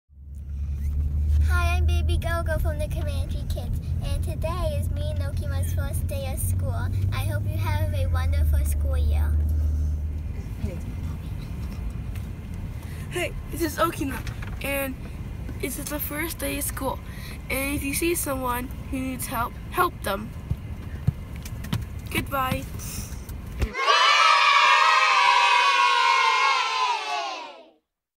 Comanche Kids- First day Of School🚌🌤🏡
See how the kids are feeling their first day back at school😁😋🙌🏽👫👬